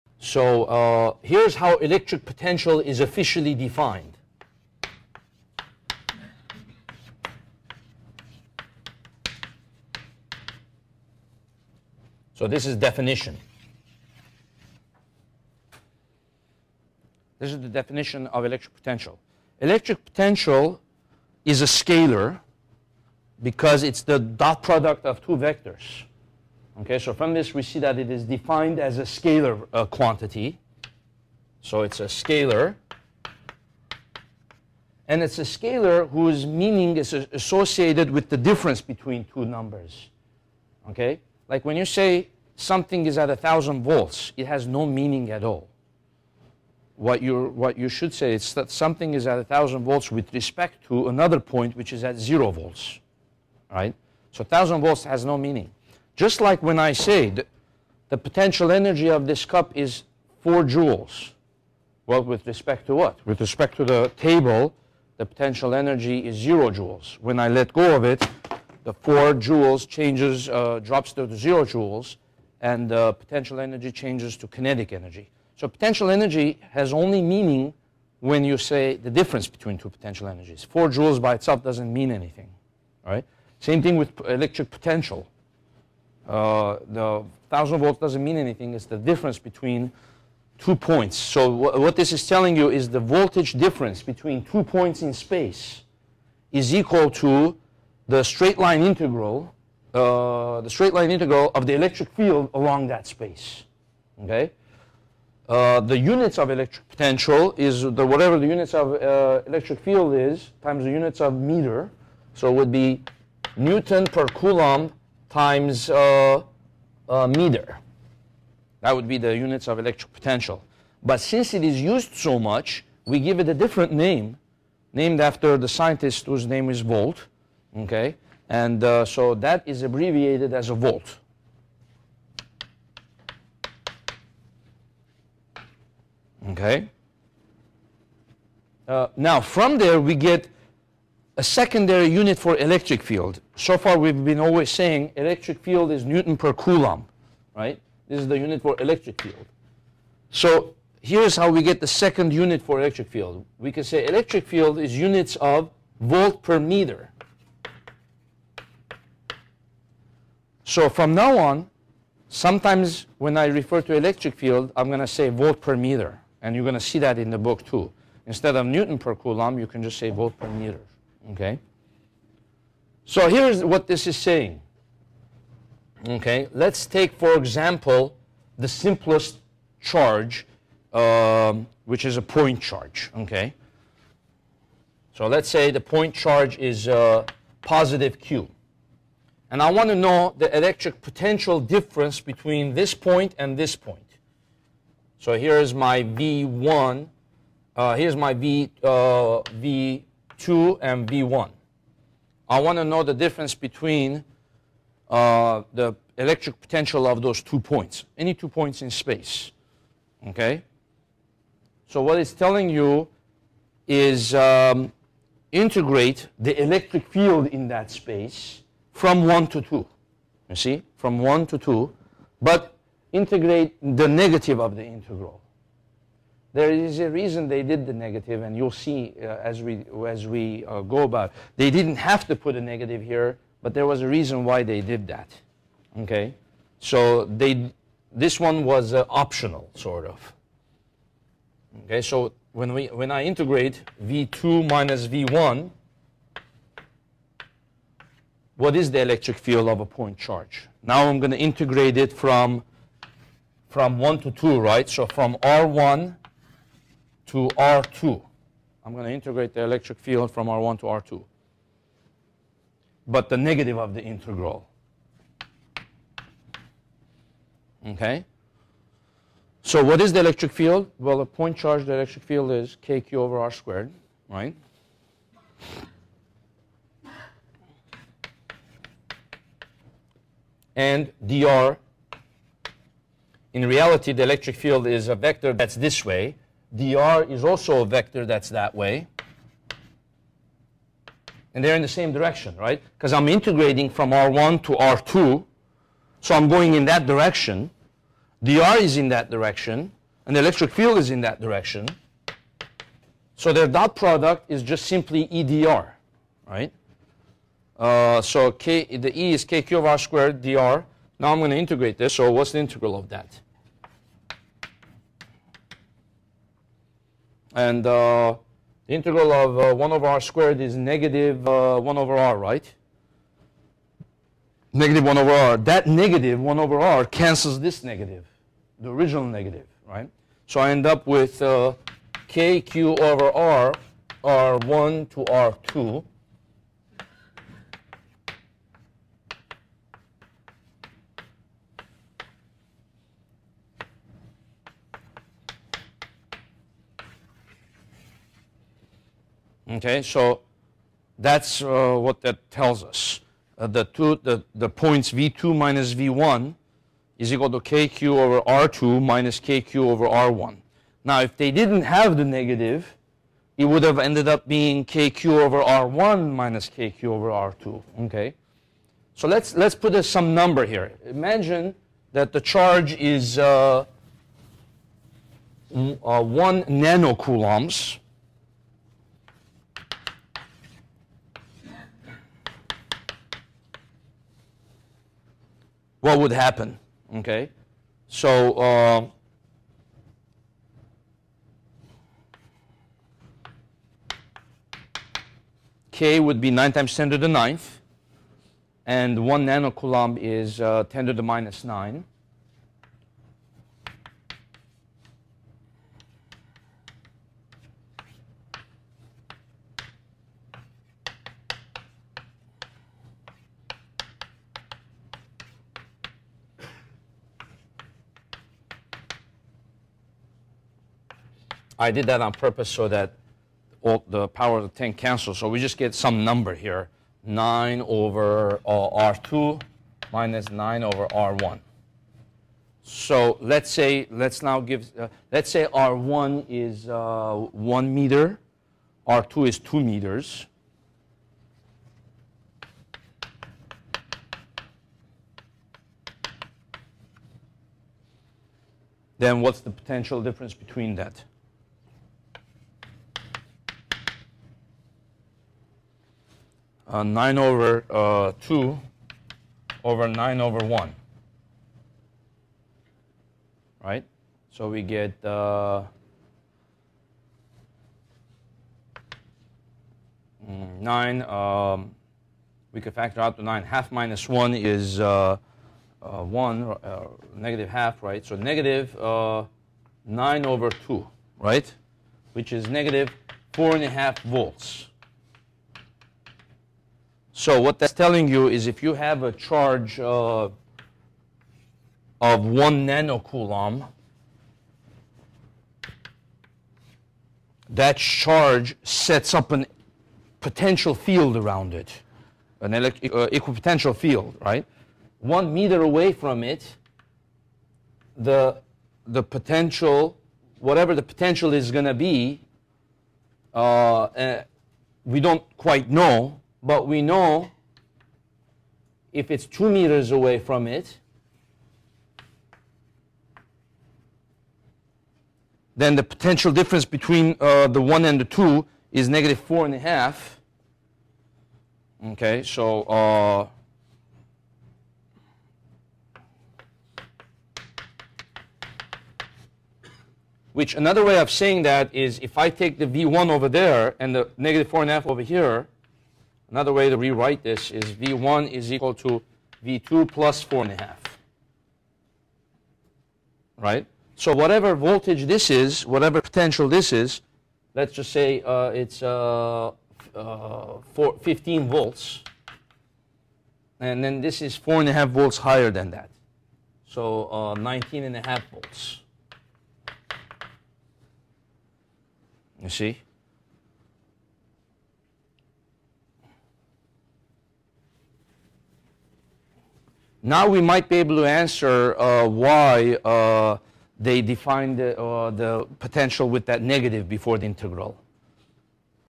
Electric Potential Theory Part 1
This clip gives the definition, meaning, and uses of electric potential.  (Part 1)